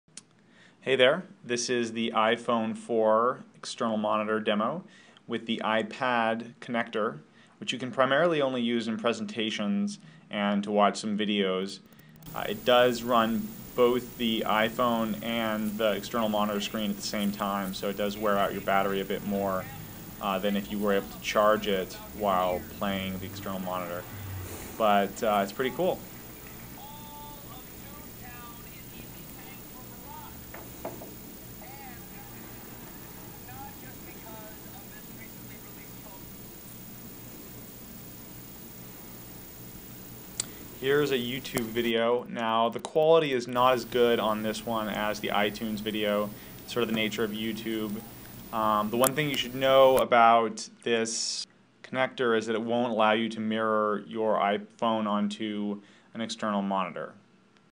iPhone 4 External Monitor via iPad VGA Adapter
The iPhone 4 can play videos and presentations on an external monitor or projector using the VGA Adapter for the iPad ($29).  The adapter has gotten lots of bad reviews because people expect it to work like a second monitor on a laptop, allowing them to mirror their iPad/iPhone onto the second monitor. While it doesn't do mirroring (probably because it would wear out the battery), it is useful for demos and presentations.